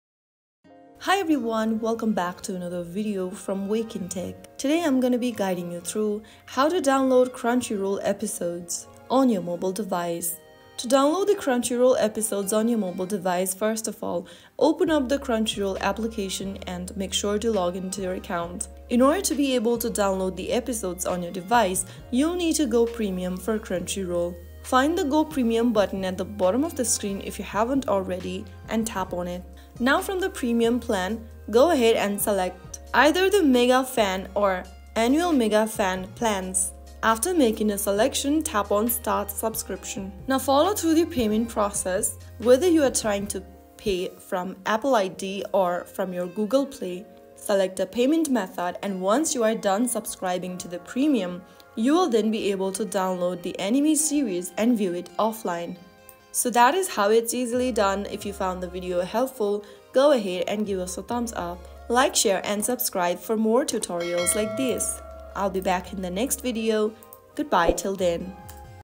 How to Download Crunchyroll Episodes? (2025 Guide) Watch Crunchyroll Anime Offline
Want to watch your favorite anime offline? In this step-by-step tutorial, I’ll show you exactly how to download episodes on the Crunchyroll app so you can enjoy anime anytime, anywhere—even without the internet! Whether you’re on Android or iOS, this guide will help you download and manage your episodes with ease.